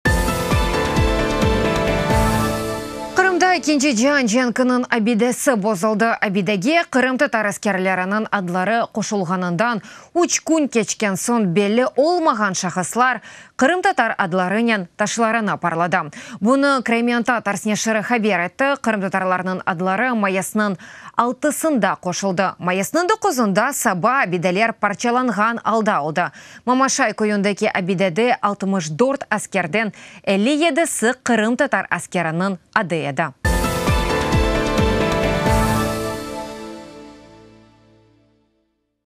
Qırımda abide bozıldı
Qırımda ekinci cian cenkiniñ abidesi bozıldı. Abidege qırımtatar askerleriniñ adları qoşulğanından  üç kün keçken soñ belli olmağan şahıslar qırımtatar adlarınen taşlarnı parladı.